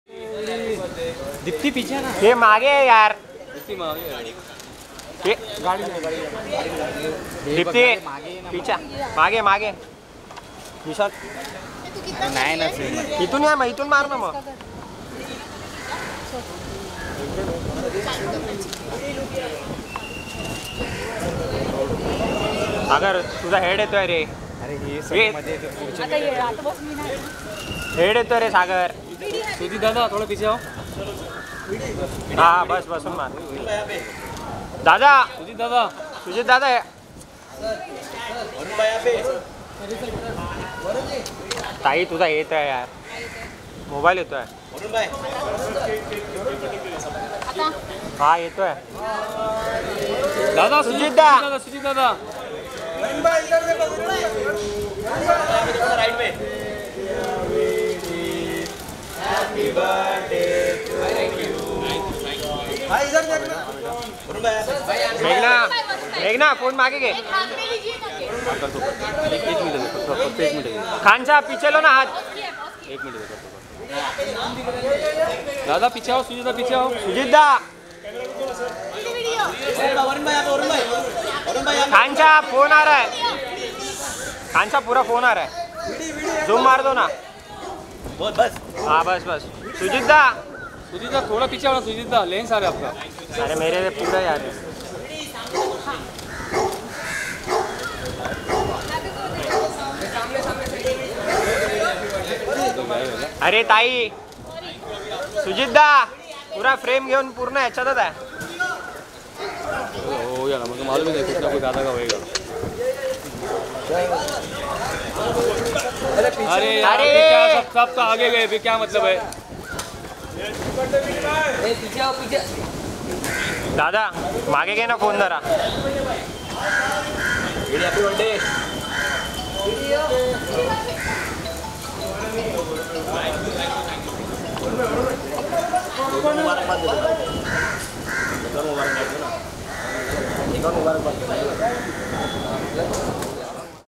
Varun Dhawan Cuts His Birthday Cakes With Media Near His Home
Baby John i.e. Varun Dhawan who turns 37 today celebrates his birthday with media, cuts three cake outside his home.